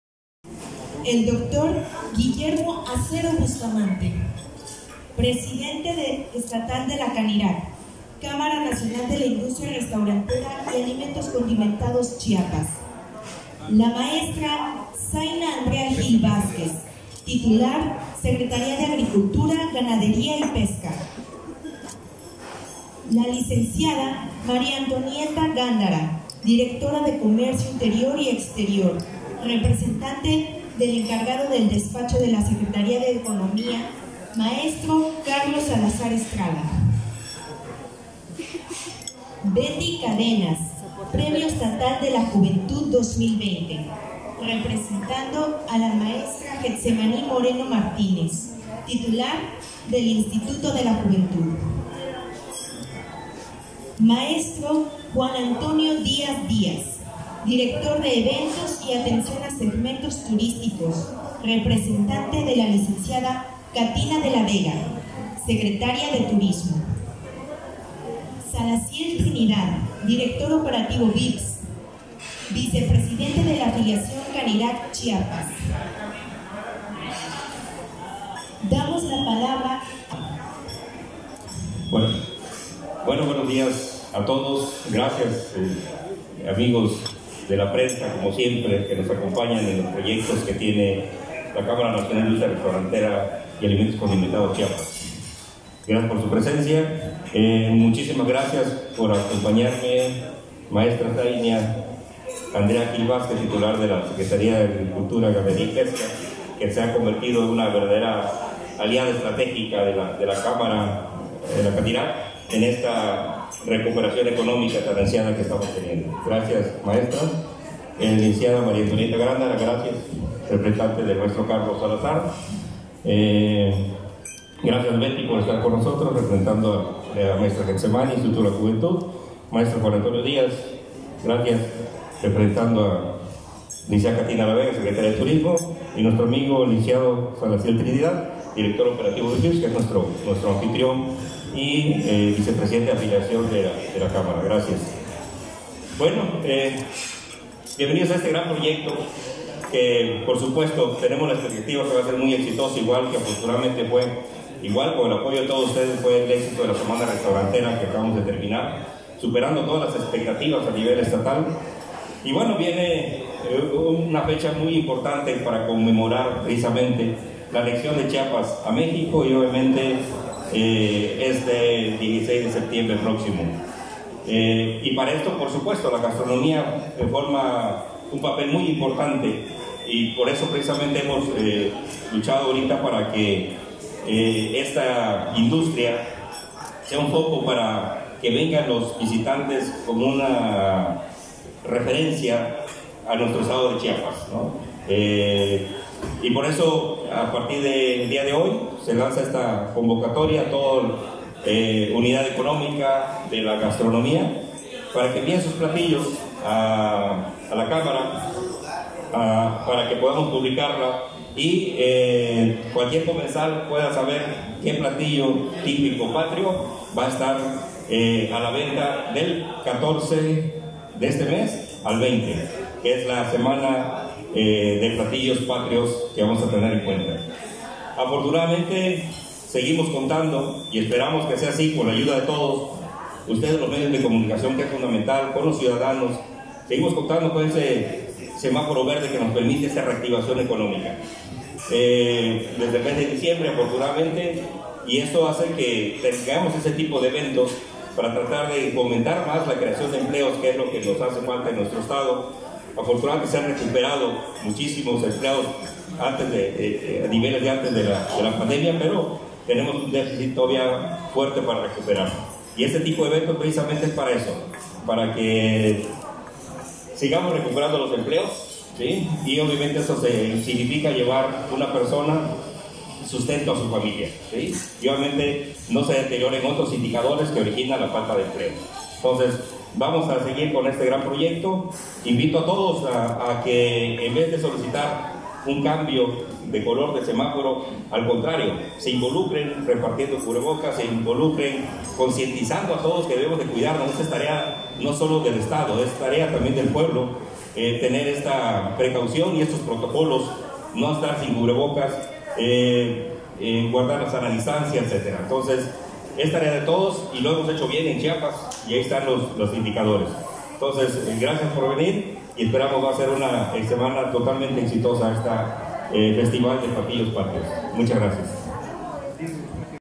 CANIRAC realizará 1er. Festival de Platillos Patrios
La Cámara Nacional de la Industria de Restaurantes y Alimentos Condimentados (CANIRAC), presenta el 1er. Festival de Platillos Patrios del 14 al 20 de Septiembre 2021